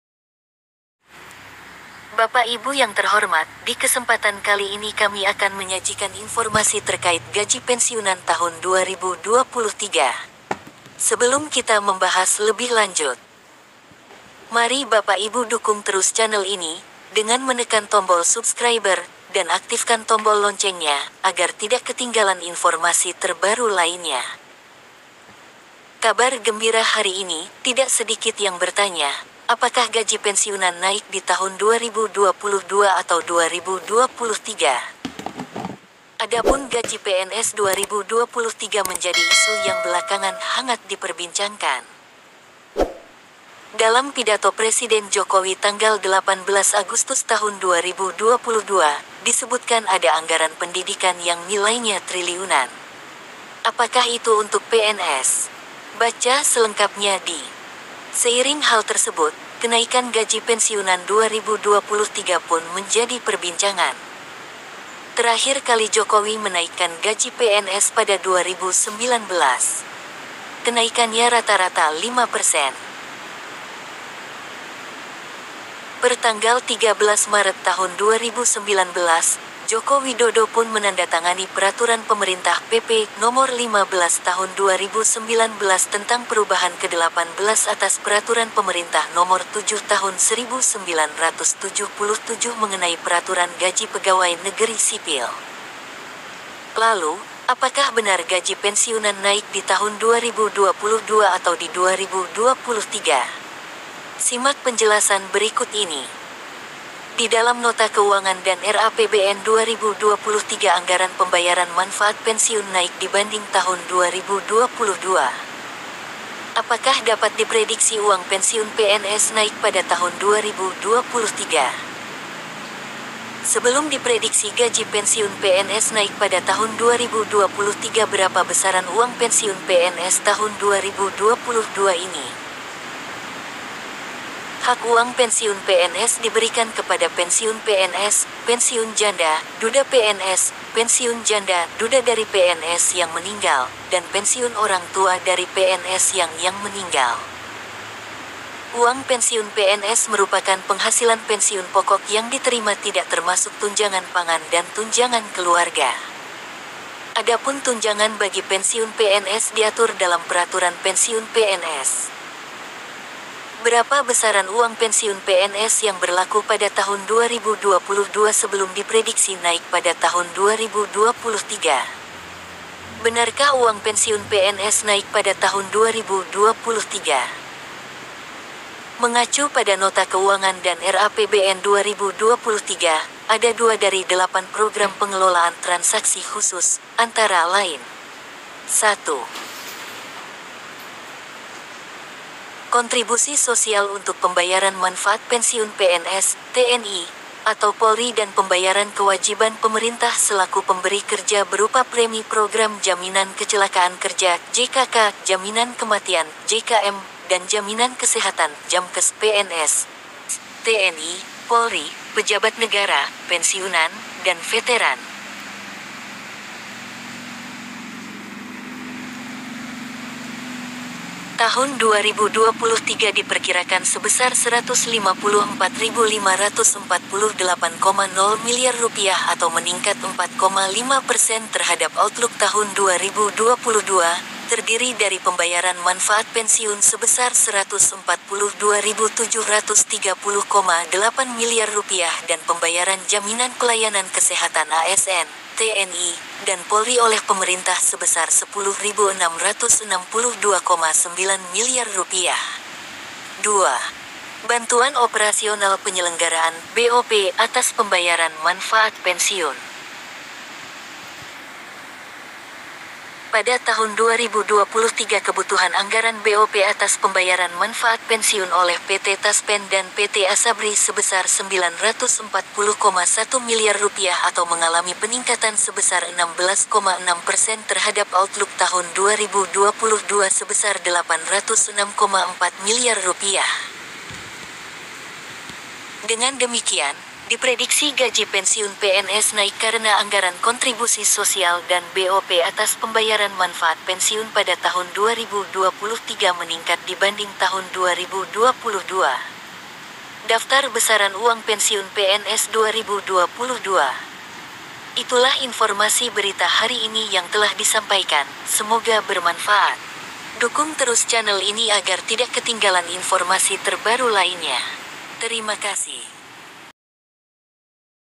INFO TERBARU.GAJI PENSIUNAN PNS 2023 NAIK DARI TAHUN INI, BERAPA PERSEN? INI BESARAN RESMI
Gaji Pensiunan PNS 2023 Naik dari Tahun Ini, Berapa Persen? Ini Besaran Resmi Terbaru.@infopnsofficial6677